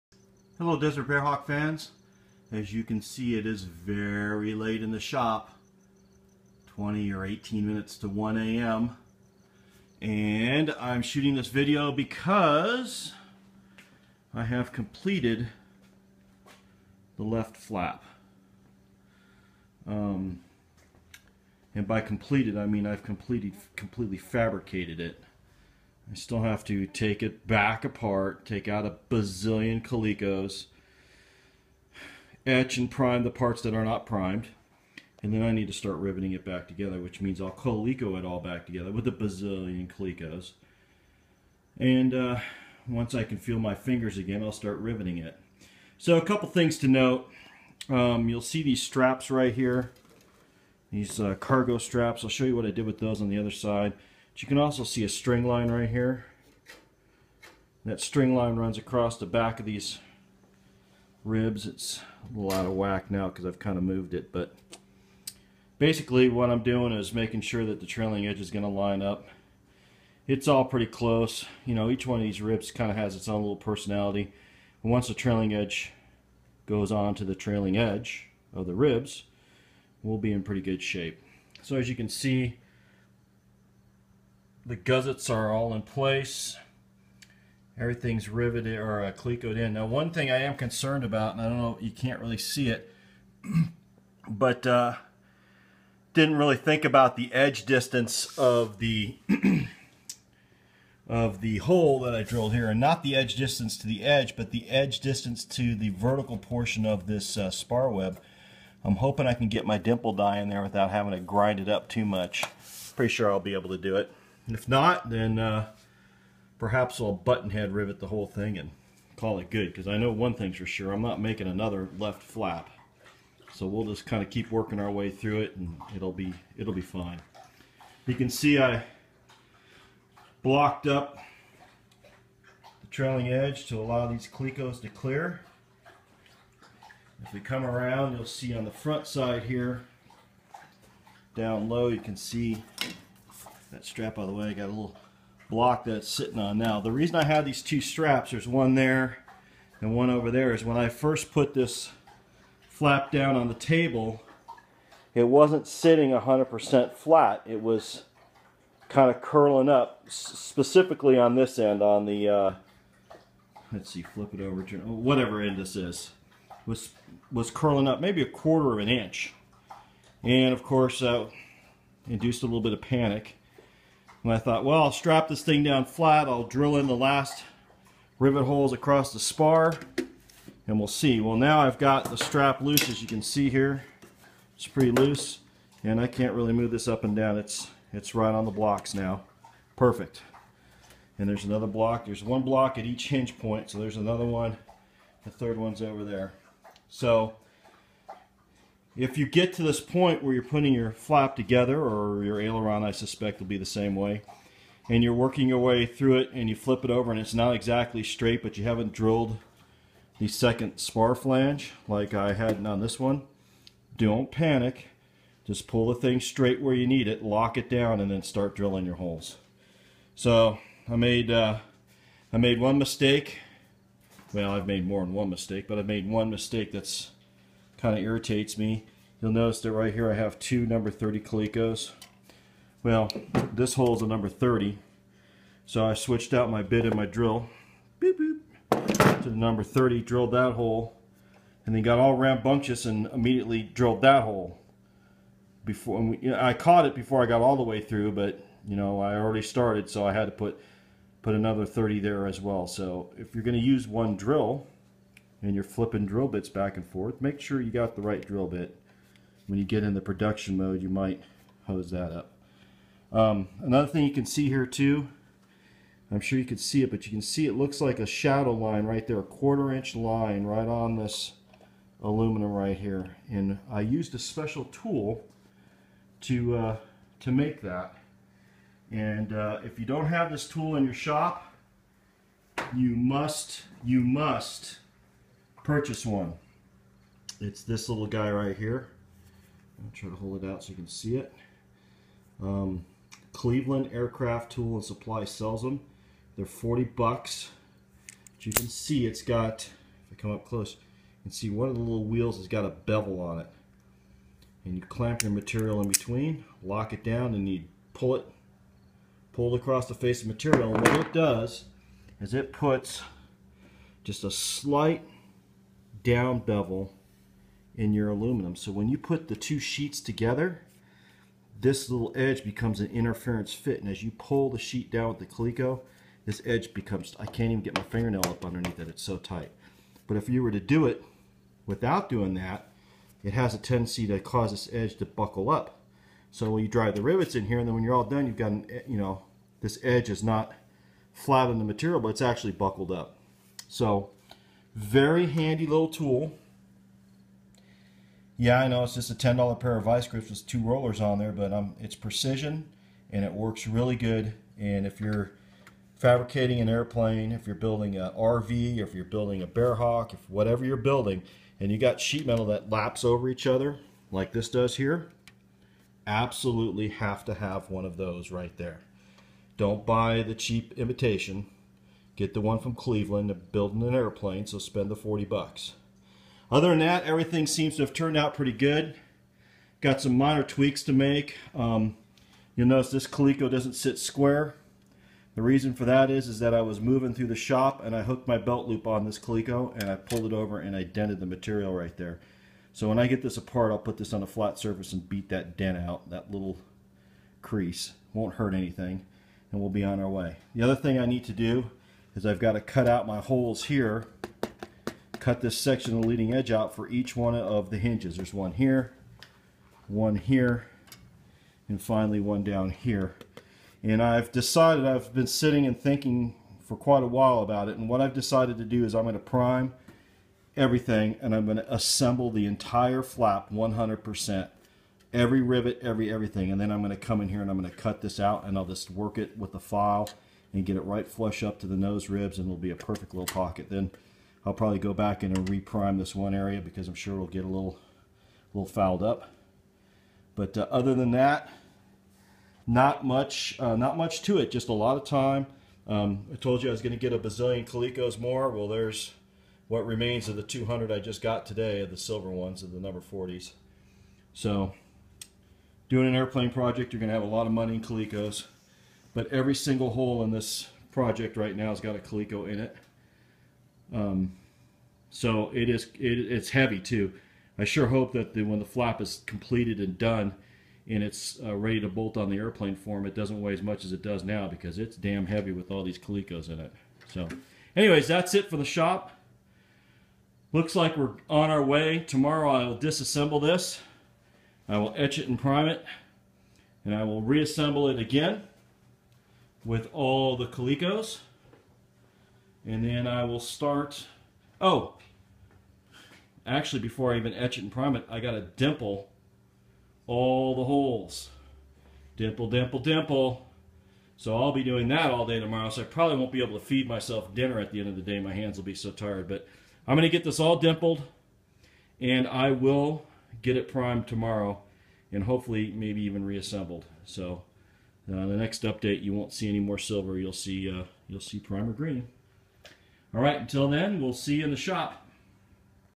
The Desert Bearhawk Project: Flap update #3
Completed the fabrication of the left flap.